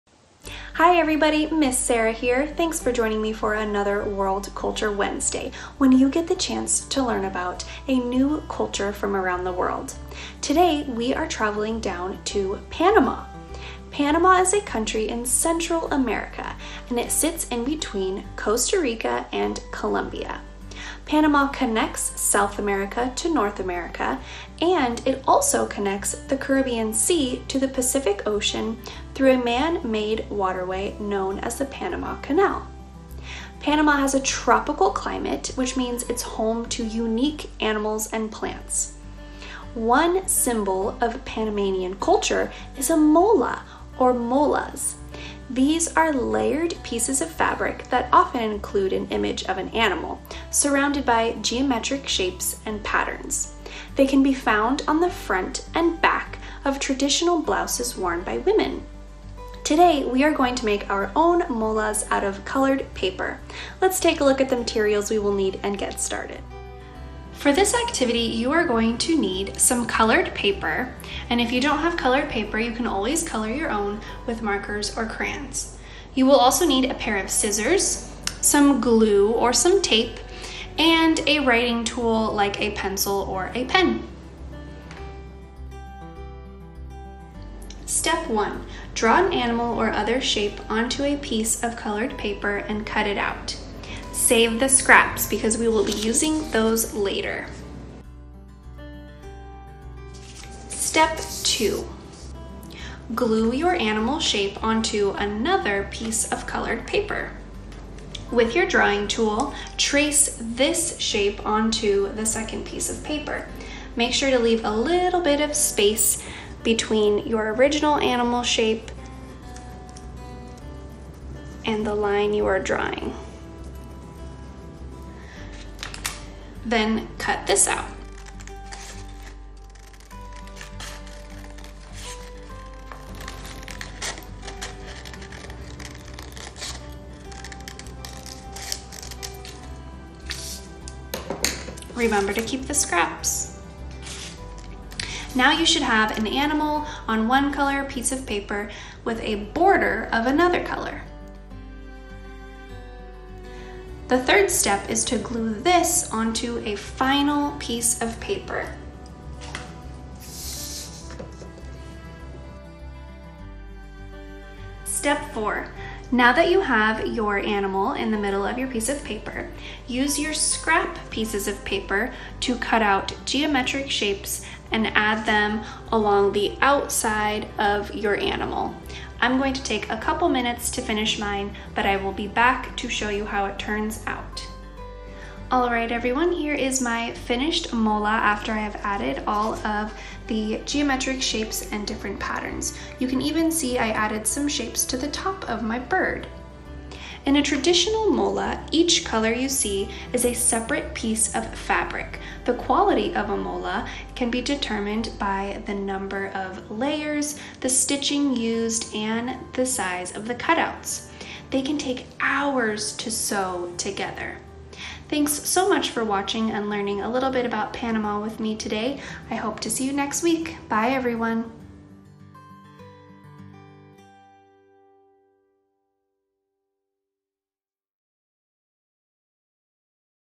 World Culture: Panama Molas
Travel to Panama with Ms. Sarah to create your own mola out of colored paper.